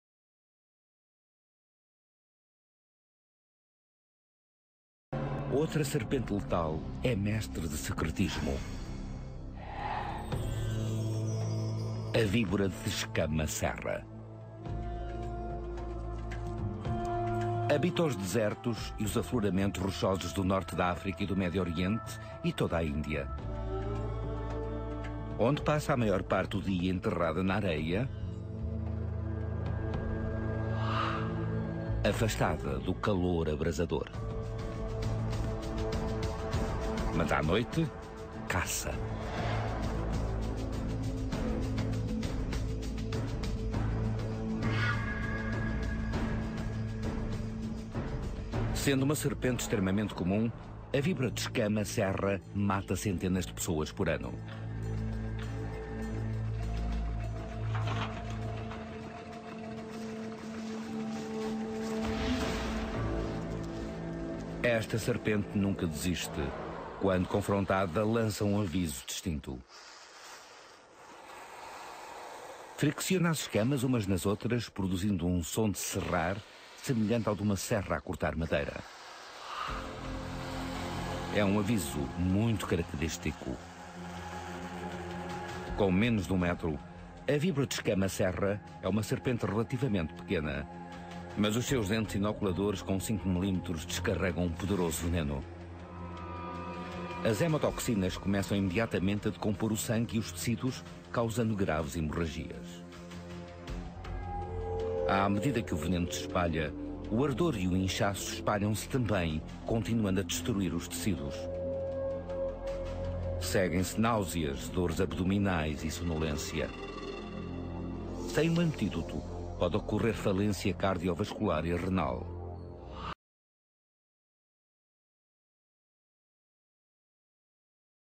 Vibora de escamas serrilhadas, 5º dos 12 animais mais perigosos da Índia
Narração em português de Portugal